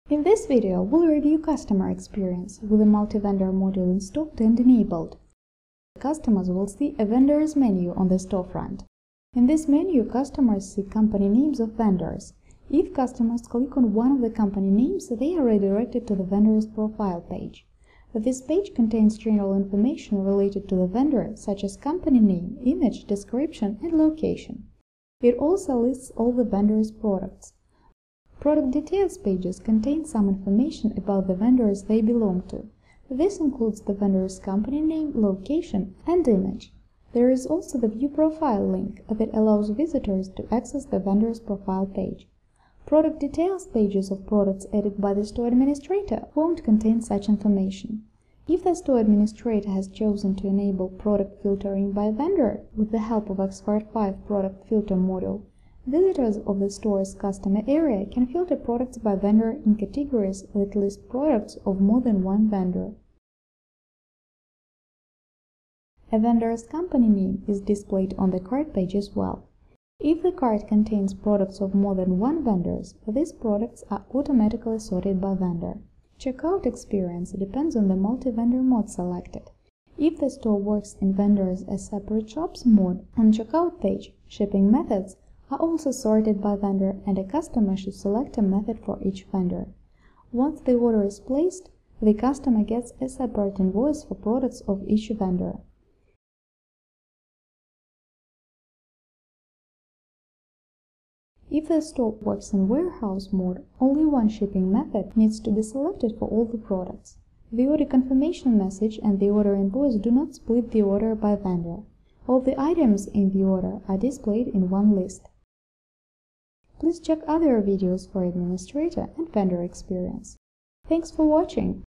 X-Cart 5 Multivendor: customer experience
This video describes customer experience in X-Cart 5 Multivendor edition:
- vendors menu
- vendors' profile pages
- vendor information
- filtering products by vendor
- checkout experience in Warehouse and Vendors as separate shops modes